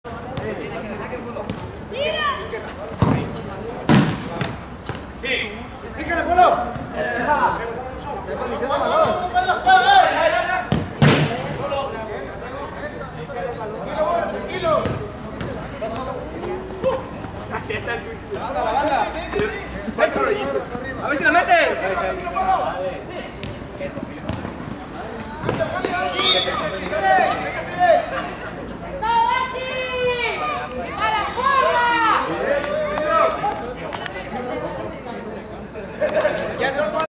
ÁGUILAS UAS VS SUSHI BORRE
ÁGUILAS DE LA UAS VENCE 11-10 A SUSHI BORRE EN EL FÚTBOL INTER EMPLEADOS UAS 2015.